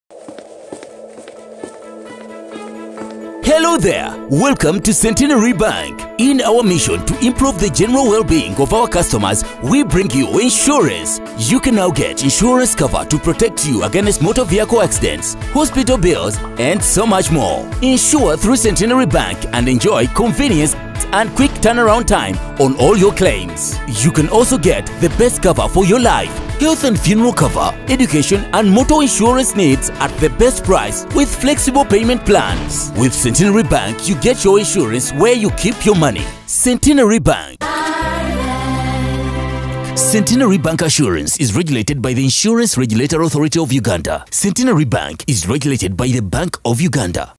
Getting Insurance through Centenary Bank - Everything you need to know
You can now get insurance cover for your health, child education, or even motor insurance through our Bank.
For further inquiry Call us toll-free on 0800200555 / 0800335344